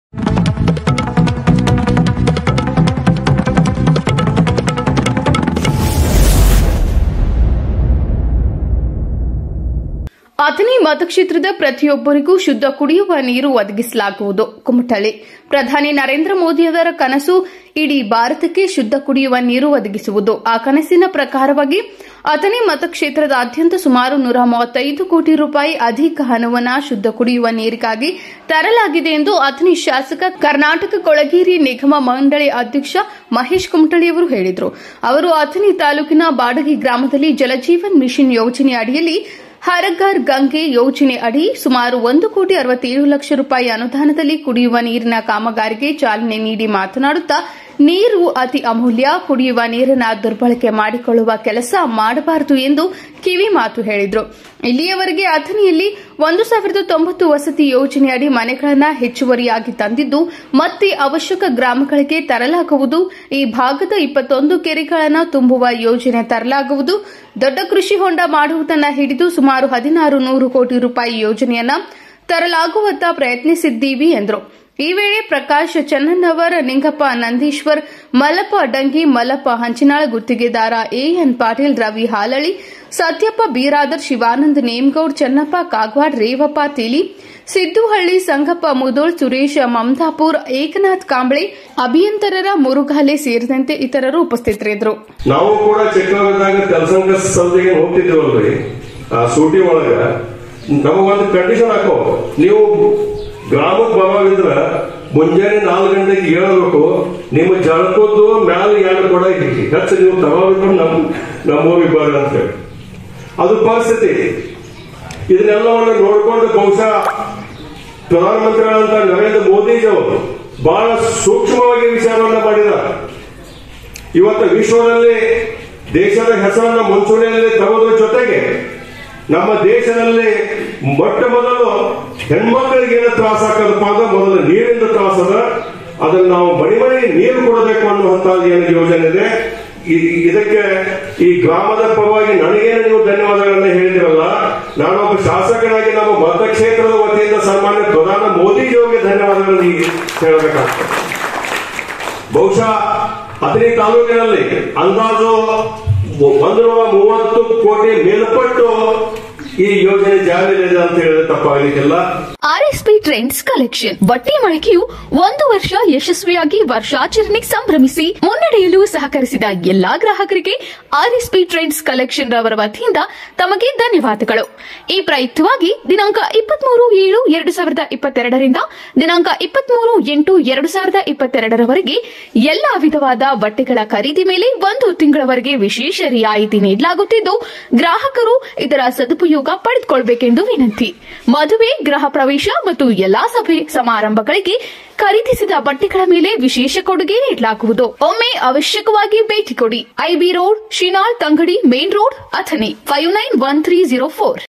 Athani: ಅಥಣಿ ಮತಕ್ಷೇತ್ರದ ಪ್ರತಿಯೊಬ್ಬರಿಗೂ ಶುದ್ಧ ಕುಡಿಯುವ ನೀರು ಒದಗಿಸಲಾಗುವುದು: ಕುಮಠಳ್ಳಿ.
ಪ್ರಧಾನಿ‌ ನರೇಂದ್ರ ಮೋದಿಯವರ ಕನಸು ಇಡೀ ಭಾರತಕ್ಕೆ ಶುದ್ದ ಕುಡಿಯುವ ನೀರು ಒದಗಿಸುವುದು. ಆ ಕನಸಿನ ಪ್ರಕಾರವಾಗಿ ಅಥಣಿ ಮತಕ್ಷೇತ್ರದಾದ್ಯಂತ ಸುಮಾರು 135 ಕೋಟಿ ರೂ ಅಧಿಕ ಹಣವನ್ನು‌ ಶುದ್ದ ಕುಡಿಯುವ ನೀರಿಗಾಗಿ ತರಲಾಗಿದೆ ಎಂದು ಅಥಣಿ ಶಾಸಕ, ಕರ್ನಾಟಕ ಕೊಳಗೇರಿ‌‌ ನಿಗಮ ಮಂಡಳಿ ಅಧ್ಯಕ್ಷ ಮಹೇಶ ಕುಮಠಳ್ಳಿ ಅವರು ಹೇಳಿದರು.

ಅವರು ಅಥಣಿ ತಾಲೂಕಿನ ಬಾಡಗಿ ಗ್ರಾಮದಲ್ಲಿ ಜಲಜೀವನ ಮೀಷನ್ ಯೋಜನೆಯಡಿಯಲ್ಲಿ ಹರ ಘರ ಗಂಗೆ ಯೋಜನೆಯಡಿ ಸುಮಾರು 1 ಕೋಟಿ 67 ಲಕ್ಷ ರೂ ಅನುದಾನದಲ್ಲಿ ಕುಡಿಯುವ ನೀರಿನ ಕಾಮಗಾರಿಗೆ ಚಾಲನೆ ನೀಡಿ ಮಾತನಾಡುತ್ತಾ ನೀರು ಅತಿ ಅಮೂಲ್ಯ ಕುಡಿಯುವ ನೀರನ್ನು ದುರ್ಬಳಕೆ ಮಾಡಿಕೊಳ್ಳುವ ಕೆಲಸ ಮಾಡಬಾರದು ಎಂದು ಕಿವಿಮಾತು ಹೇಳಿದರು.

ಇಲ್ಲಿಯವರೆಗೆ ಅಥಣಿಯಲ್ಲಿ 1090 ವಸತಿ ಯೋಜನೆಯಡಿ ಮನೆಗಳನ್ನು ಹೆಚ್ಚುವರಿಯಾಗಿ ತಂದಿದ್ದು ಮತ್ತೆ ಅವಶ್ಯಕ ಗ್ರಾಮಗಳಿಗೆ ತರಲಾಗುವುದು, ಈ ಭಾಗದ 21 ಕೆರೆಗಳನ್ನು ತುಂಬುವ ಯೋಜನೆ ತರಲಾಗುವುದು, ದೊಡ್ಡ ಕೃಷಿ ಹೊಂಡ ಮಾಡುವುದನ್ನ ಹಿಡಿದು ಸುಮಾರು 1600 ಕೋಟಿ ರೂ ಯೋಜನೆಯನ್ನು ತರಲಾಗುವತ್ತ ಪ್ರಯತ್ನಿಸ್ತಿದ್ದಿವಿ ಎಂದರು.